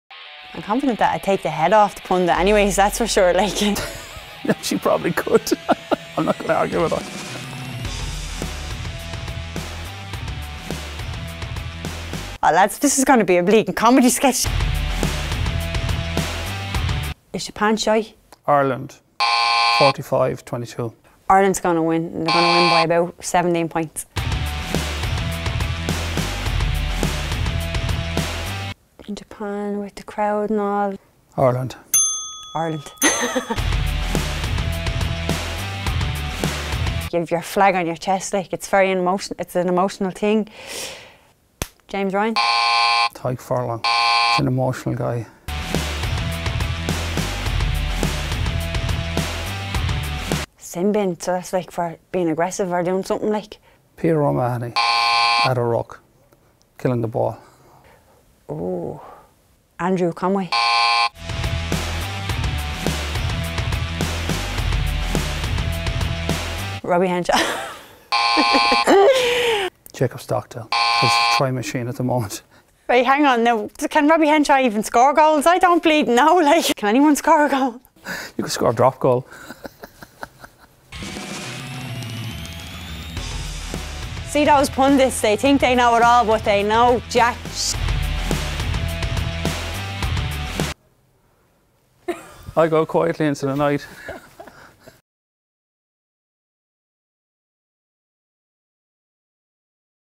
Punter and Pundit are as shocked as the rest of us!
Eddie O’Sullivan and Kellie Harrington go head to head for Ireland's second game against Japan in the Rugby World Cup.